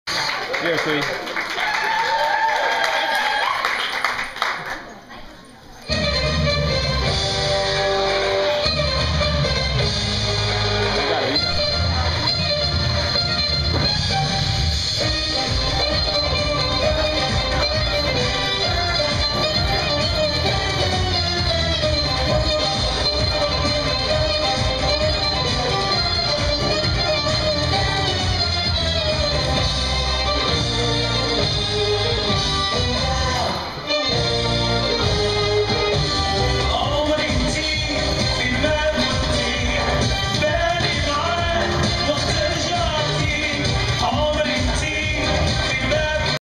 Vagabond Bellydance @ Elgin Holiday Hafla
Vagabond Bellydance @ Holiday Hafla for the Nilaja surgery fund! Martini Room in Elgin. 12/23/10.
Deserae Hunyar and Beloved Freya